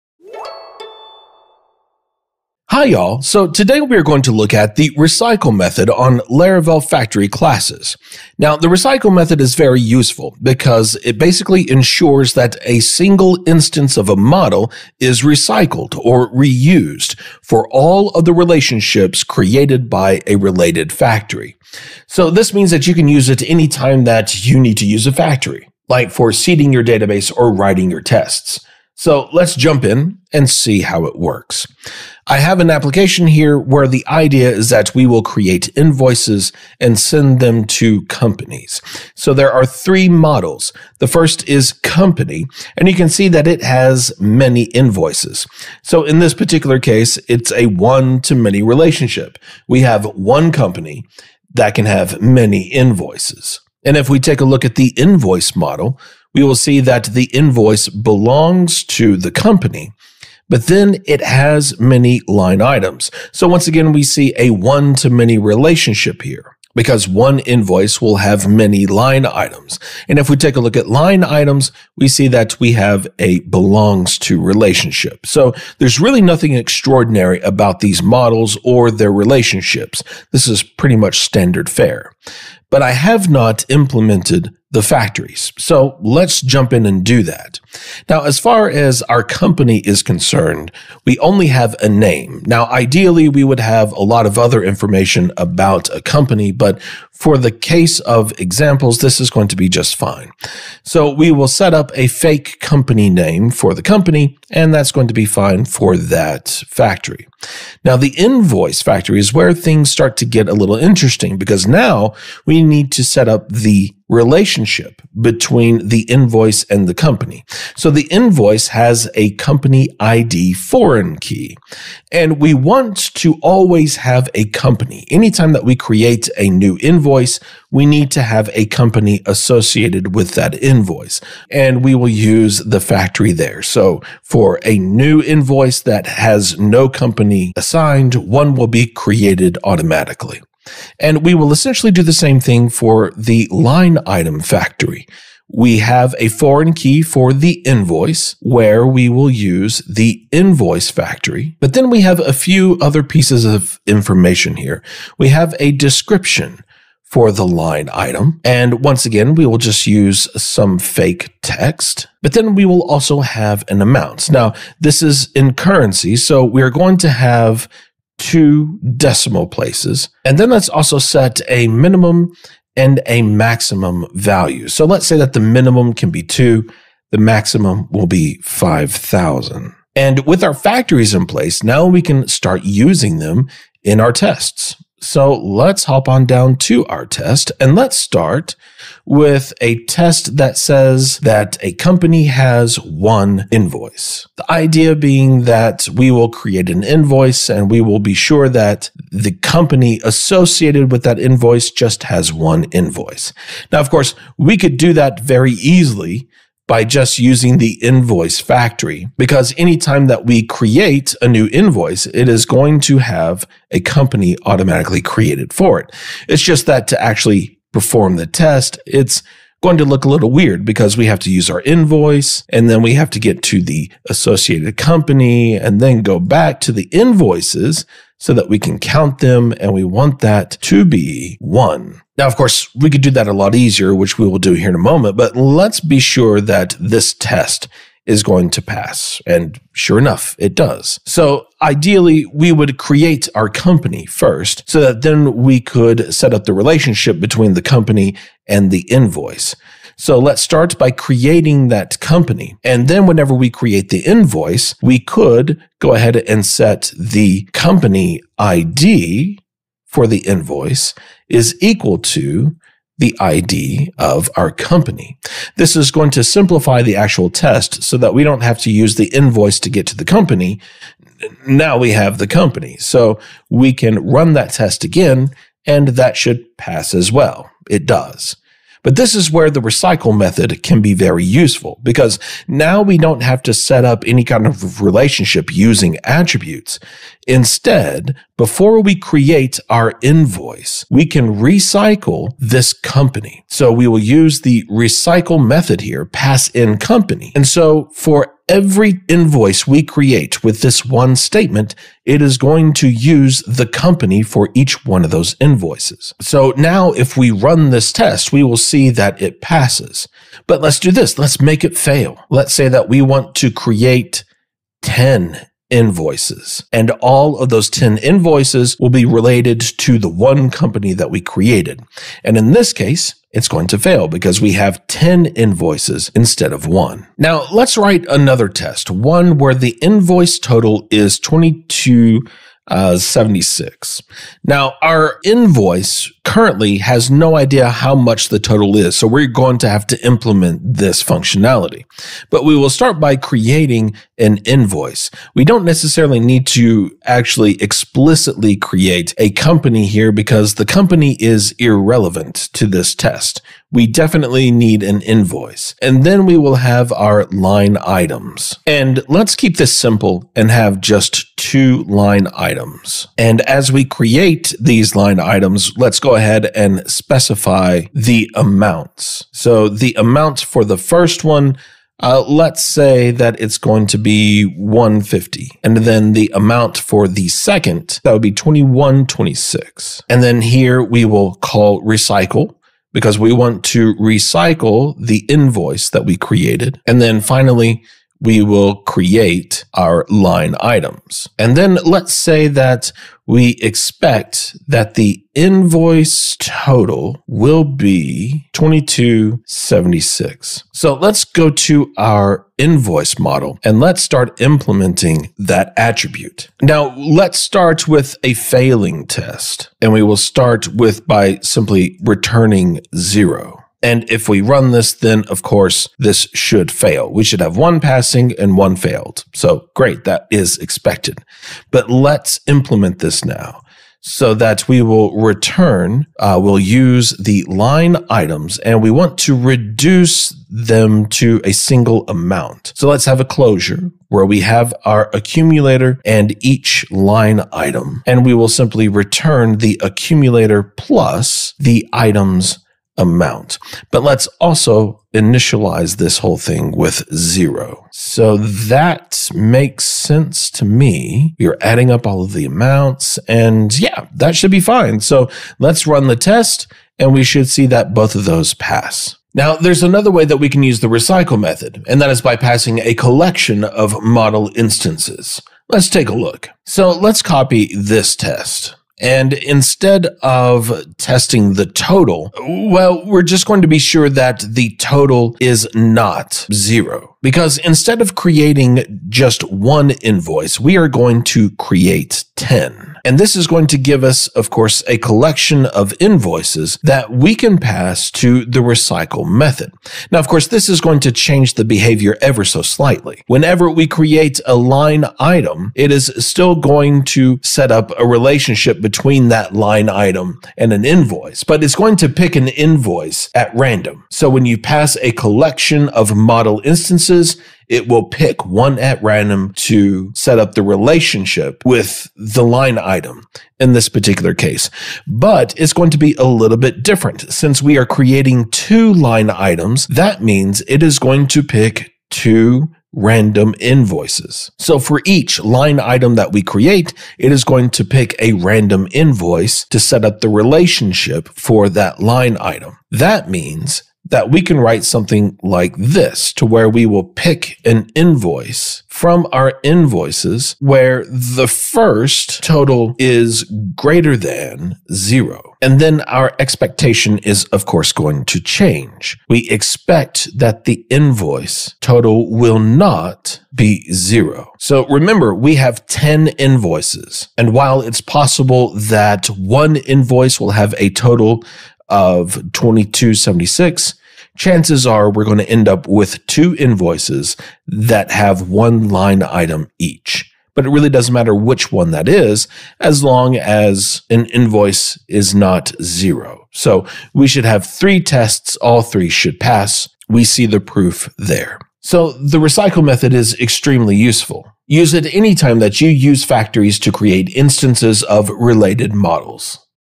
Jeremy McPeak's Larabits - How to Recycle Laravel
Frustrated with tying related model instances together when using your factories? Laravel's recycle() method makes it incredibly easy to create related model instances with your factories. It can be confusing to describe, so let me show you how it works in this Larabit.